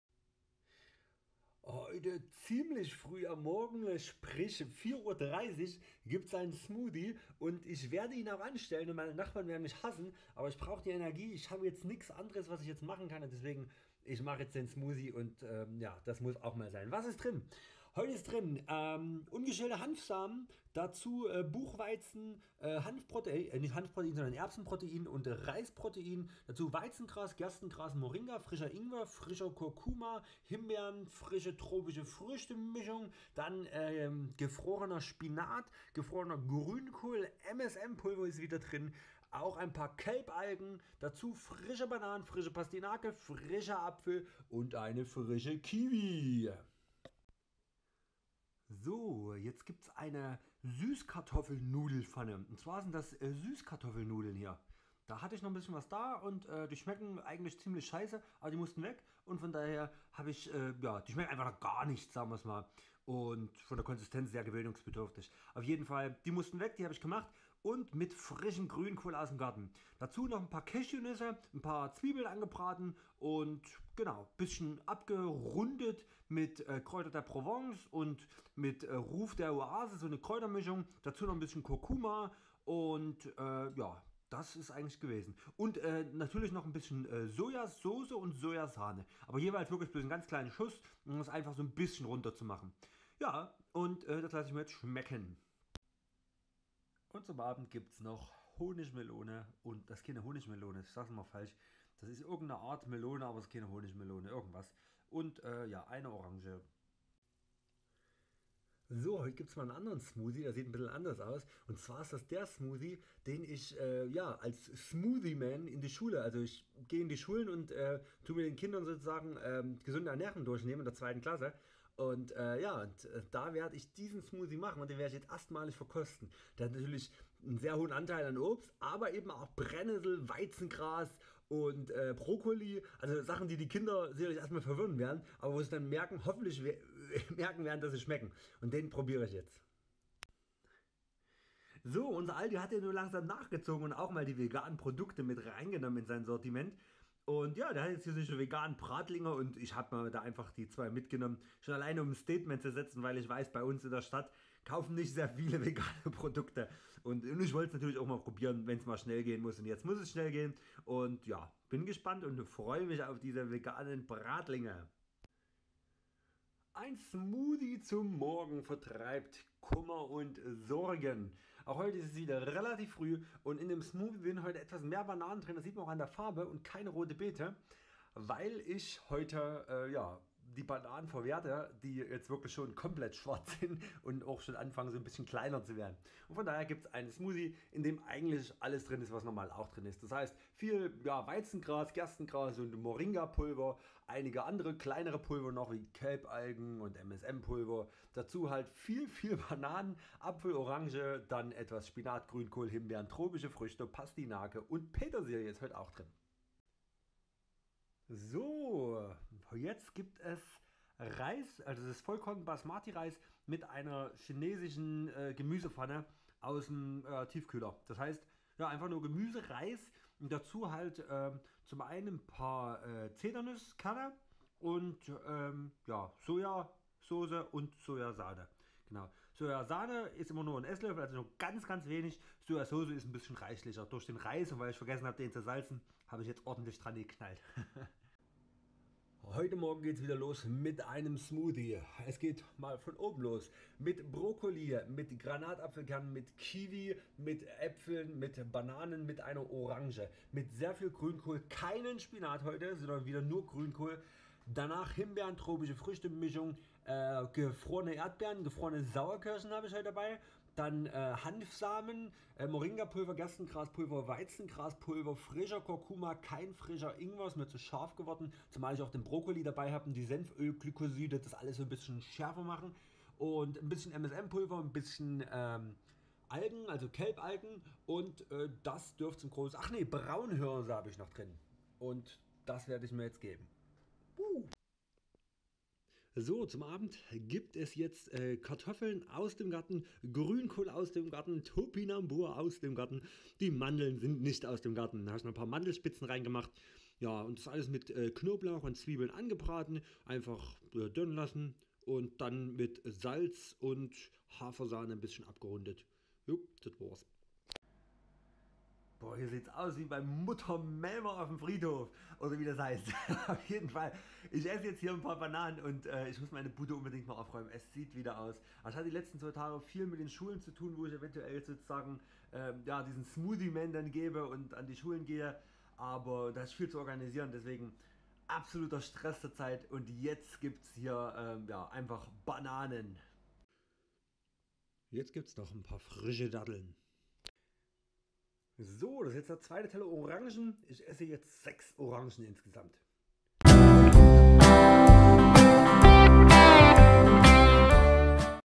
Food Diary deutsch, eine Woche vegan & gesund und viel Rohkost! echtemahlzeiten Woche 36
Echte Mahlzeiten! Jede Woche neu! Food Diary deutsch, vegan & gesund und viel Rohkost! Essen satt. Einfache gesunde leckere Mahlzeiten. Link zum preiswerten Mini Mixer real life! Folge 36! Meine Webseite